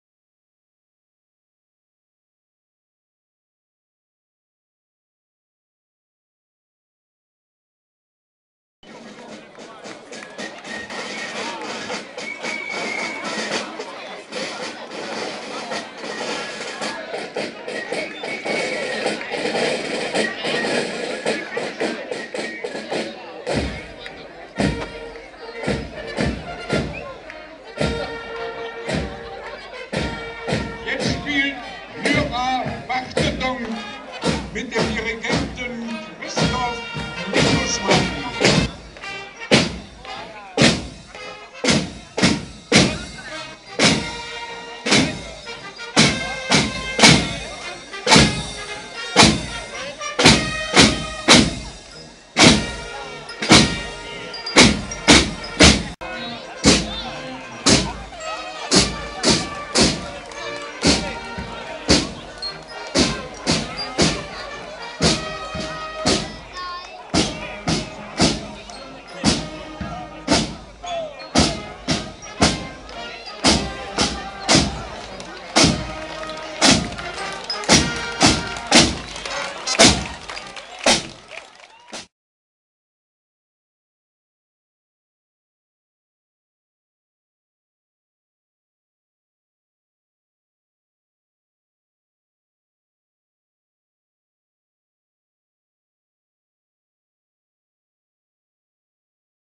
jüngster Tambourmajor Luca Schöler 9 Jahre - Tambourkorps Schwalmtal-Amern Parade in Grefrath-Oedt
Das Tambourkorps Schwalmtal-Amern spielte im Sept.2013 die Parade in Grefrath-Oedt unter der Leitung von Luca Schöler (9 Jahre) - jüngster Tambourmajor.
Mitwirkende Spielleute: Michael Messinger, Miriam Knüppel-Caspers, Hannah Bischofs, Heiko Stuffertz, Jens Fiethen, Martin Sartingen, Lars Rauchfuß, Sebastian Hurtz, Jannik Mackes, Bernd Toerschen, Andreas Rütten